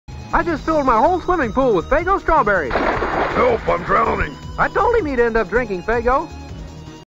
[S] Gamzee: Make a Faygo ad.
Happy Hiveswap Act 2 Release Date's Release Date! To celebrate, I took one of those Wilkins ads for a non-Wilkins product, and made it a Homestuck.